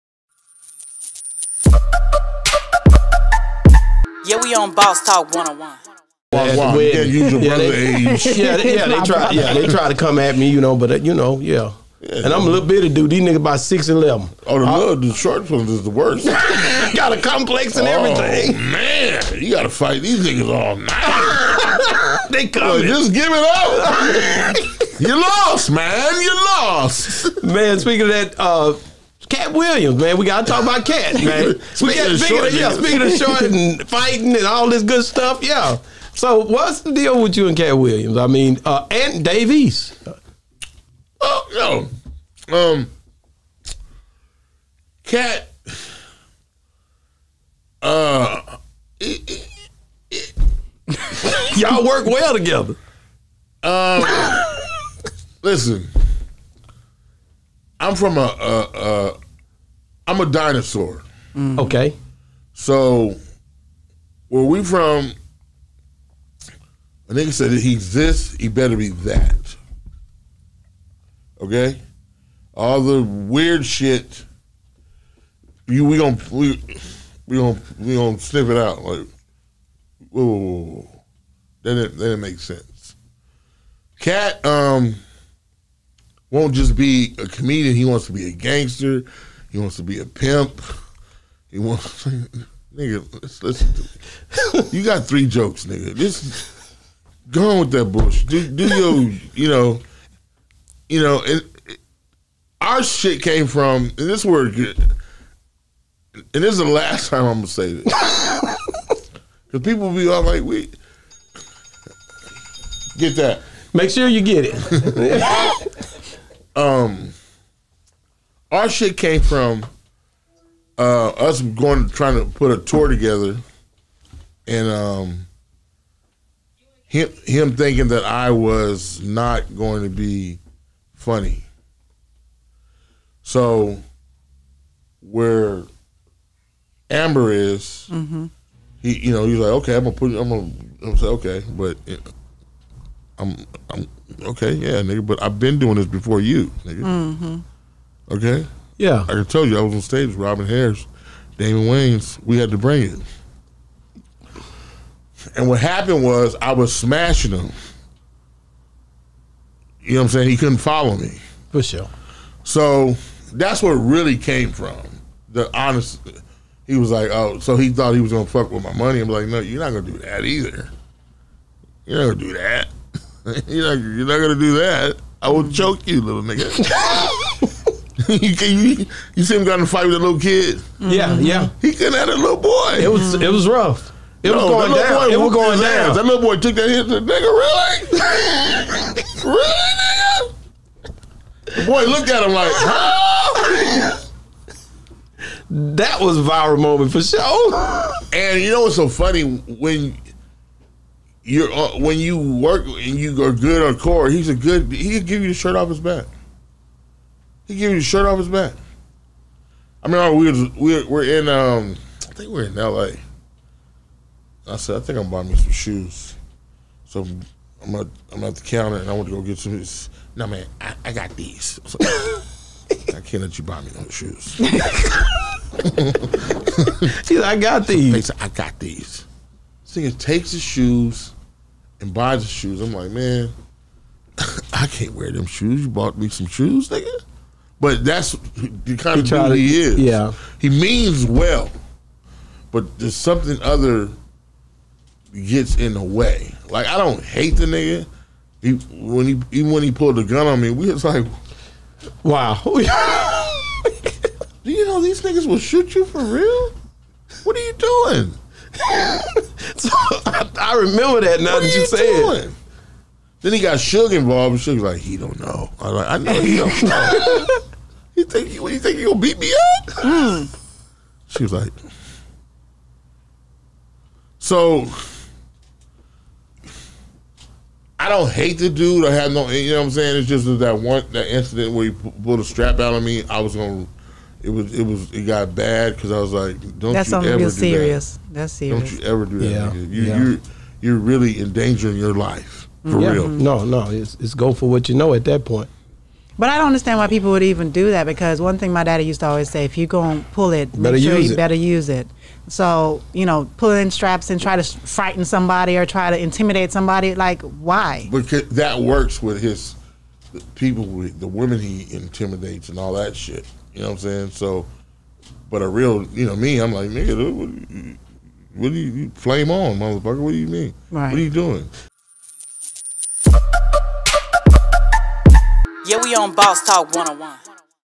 Faizon Love on Katt Williams Pulled a Gun on me Suge Knight tried to tell him | Katt Willams Tripped
Faizon Love on Katt Williams Pulled a Gun on me Suge Knight tried to tell him